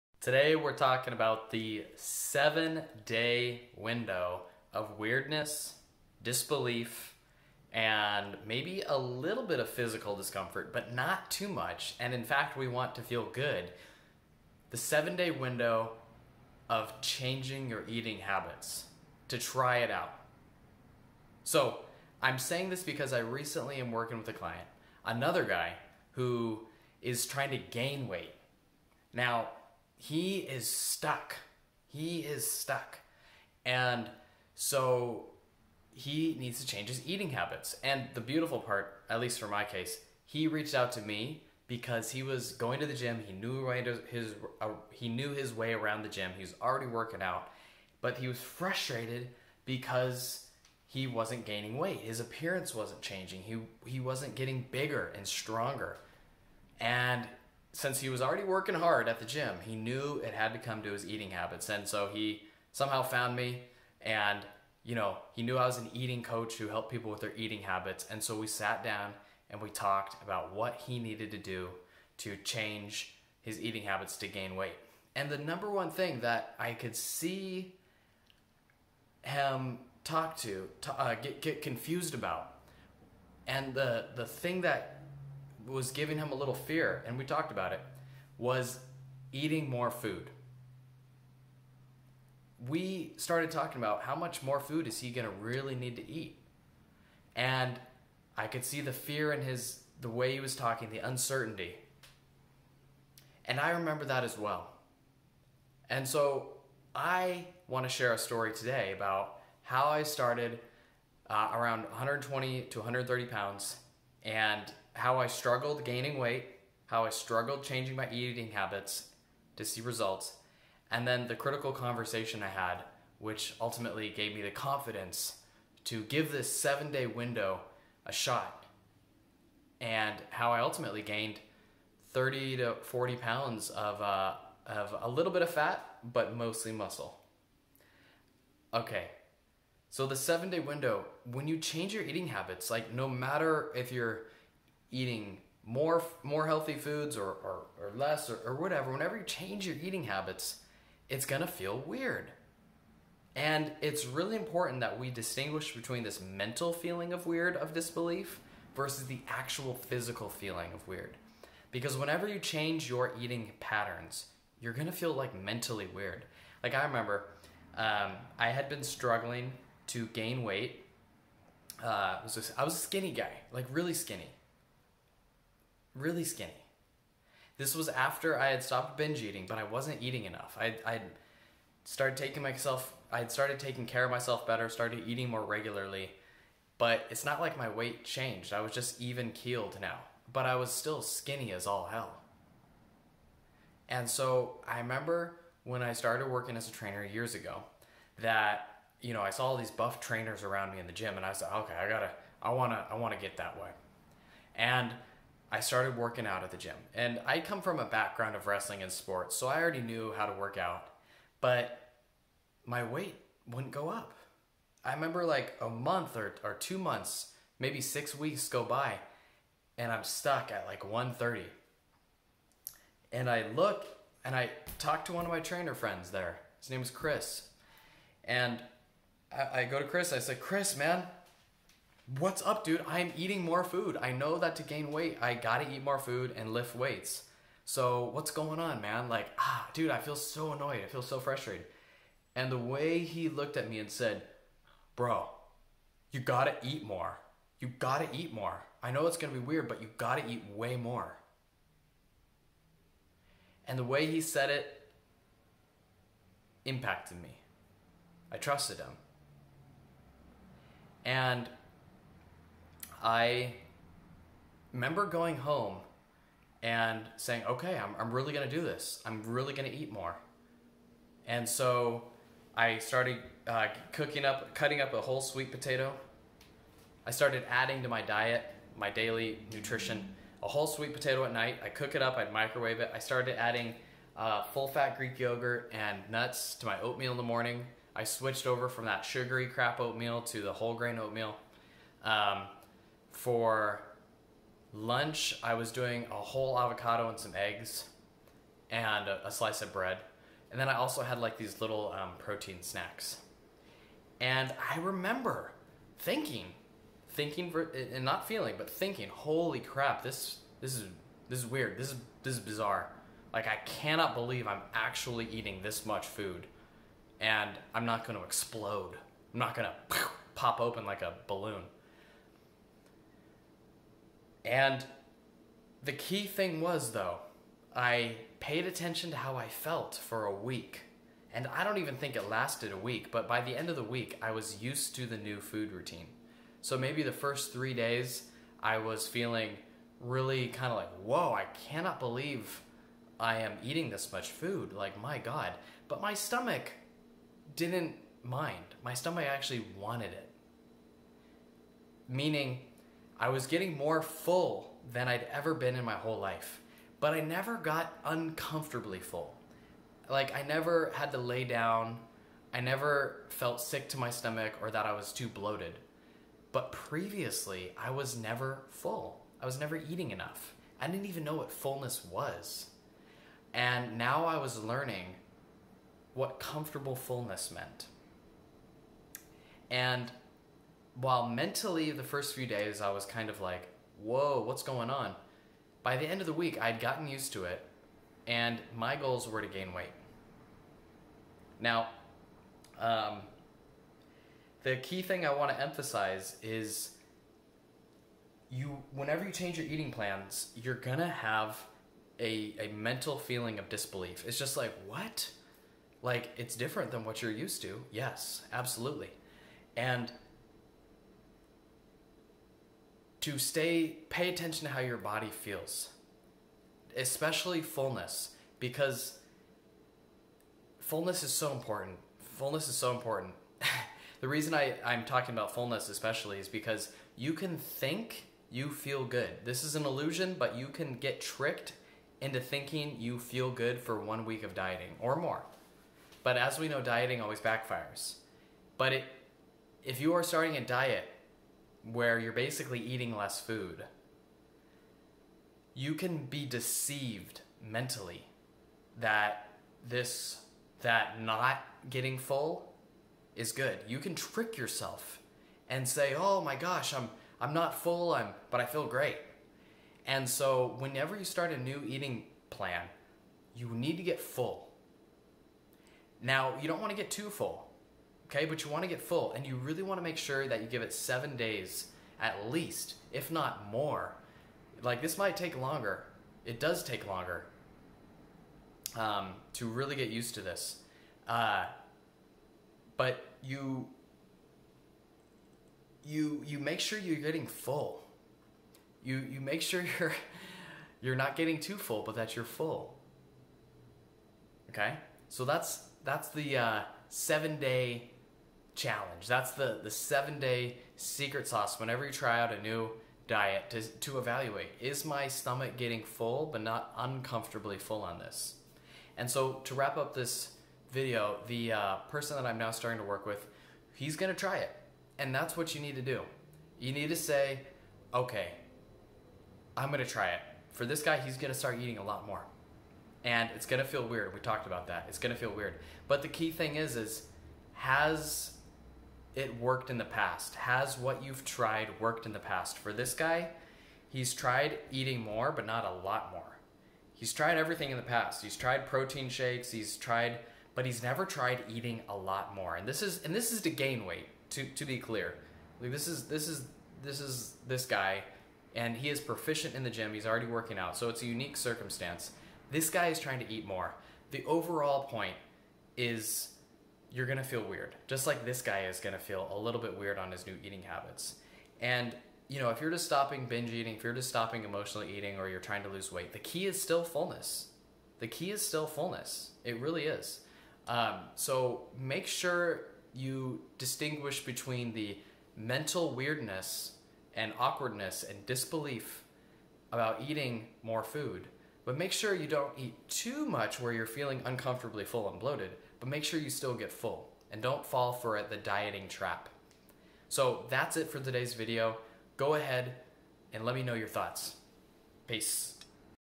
How Much Do I Need To Eat To Gain Weight? Give Seven Days Eating More Challenge A Good Shot
Let's talk about eating routines and why the number 7 is extremely important.

Seven means seven days. 7 days to try out a new eating routine and see if it feels good.

Notice how I say feels good, not works.

You aren't going to know in 7 days whether and eating plan works or not. 7 days isn't enough time to gain weight, lose weight, or stop binge or emotional eating.

So you can't judge your new eating plan based on results in seven days.

However, you can evaluate your eating plan in 7 days based on how you feel.

Are you willing to try something new for 7 days?

Commentary:

We want to have seven days to try out an eating plan because whenever you try out and eating plan, your belly is going to feel a little awkward. And I say this because I have a client right now who is trying to gain weight.

We are working on his eating habits, because that's what I do as an eating coach, I work with people's eating habits. And this guy is trying to gain weight. He's trying to get more muscle specifically.

Now in this case, this guy is already going to the gym and knows his way around the gym. He reached out to me because he felt incredibly frustrated that he couldn't gain weight or muscle mass. He's what you might call skinny. And he's trying really hard to gain weight, gain muscle, but it just hasn't worked.

So he's realizing that the answer is in how he eats. And I can relate because I used to be incredibly skinny as well. I used to be 120 lbs and now I'm around 160.

So I know skinny can oftentimes seem desirable, but for many people such as myself and that current client I'm working with, being too skinny is just not what we want.

And I just wanted to share a story of mine from when I made the transition from being too light to finally actually gaining weight. Because for 6 to 8 weeks I forget the exact number but for some amount of time I was actively trying to gain weight, I was going to the gym and I thought I was eating more.

However, my weight didn't change. I was getting stronger, but my weight wasn't going up my size wasn't growing. And I remember this moment when I had a conversation with a fellow personal trainer.

He looked me in the eye and said it's going to be uncomfortable for 7 days or so when you really get used to eating more food. And the way he looked at me in the eyes and the way he said it I really took it to heart.

So I went home, and I started eating more food. I started eating sweet potatoes at night for the extra carbs. I need sweet potatoes or a whole grain carb and we're healthy. I started loading up full fat Greek yogurt and nuts and whole grain oatmeal for breakfast.

For lunch I started having eggs and avocado, along with whole grain bread. And knowing that I was going to be uncomfortable for 7 days helps me a lot. Because I remember feeling shocked and in quasi disbelief that I had to eat this much food. It was like this much food? This is going to work? I can't believe it!

And it felt weird eating so much food simply because I wasn't used to it. Now I want to be clear here - I am not recommending that you feel bloated.

 You might think that you have to eat until you feel uncomfortably full. But we never want that. So Don't think that you have to eat too much to wear that you feel bad. When I say that I felt weird and in disbelief I just remember my stomach was actually full.

 Previously my stomach was never full. And never got really satisfied. So I remember my stomach would be very full, but it wasn't like I felt sick to my stomach because I ate too much. It was just that I felt full. And after 7 days or so, I started getting used to it. It wasn't weird anymore.

I was no longer afraid of being full. And that's when I gained 30 to 40 lb.so if you are out there and you're trying to change your eating patterns give it a week and really evaluate in terms of hunger and fullness.

And see how you feel. We want fullness. Whether you are losing weight whether you are gaining weight whether you are stopping binge eating we want you to be comfortably full. so just know that it might take your belly And mind a week or so to get used to the new patterns.

And it might take longer than 7 days too, but just don't rush to conclusions. Really take your time to feel under your body. Make sure your body feels good. But expect a little weirdness in the beginning.